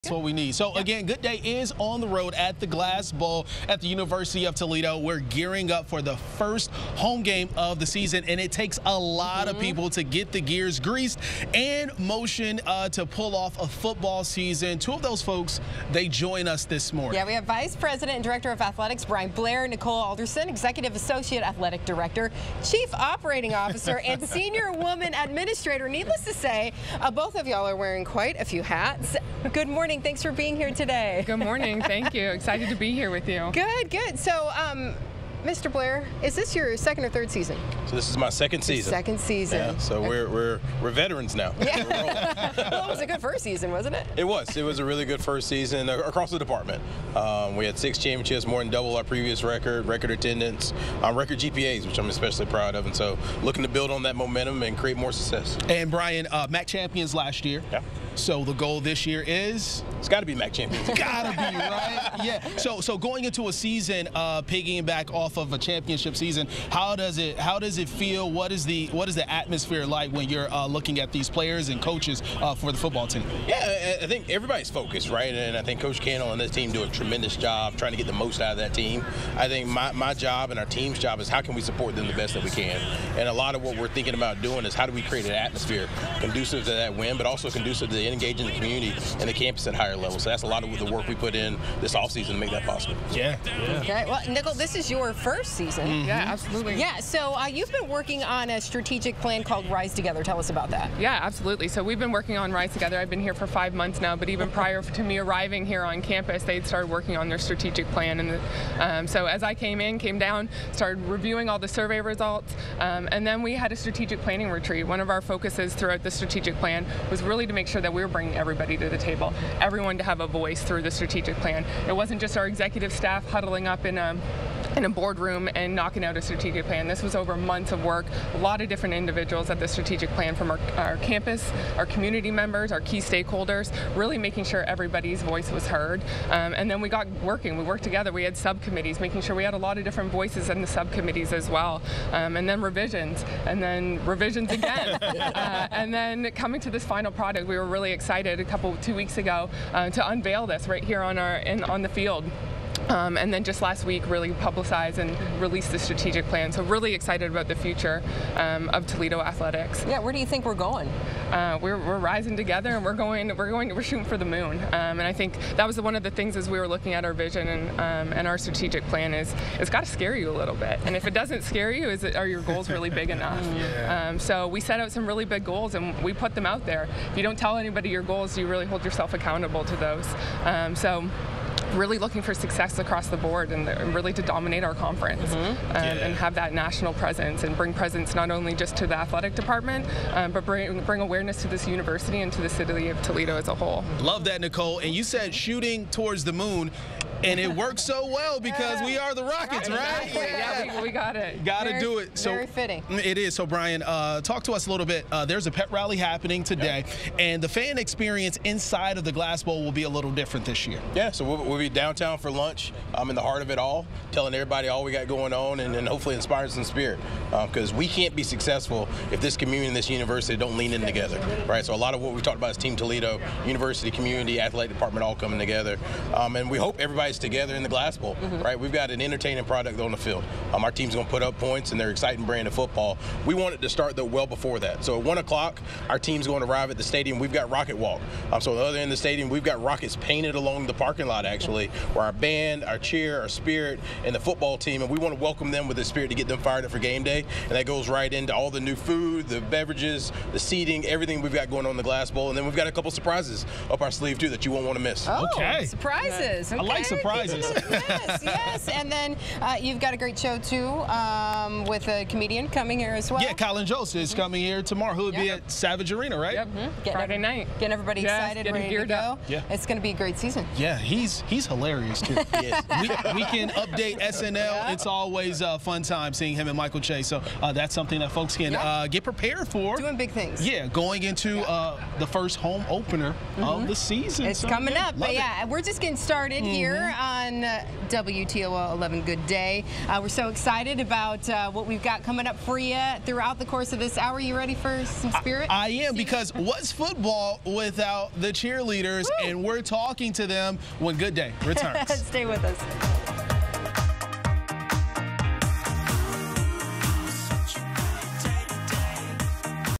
UToledo gearing up for first home game of the season | Good Day on the Road
Vice President and Director of Athletics Bryan Blair and Nicole Alderson Executive Associate Athletic Director talk the strategic plan for UToledo Rockets Football.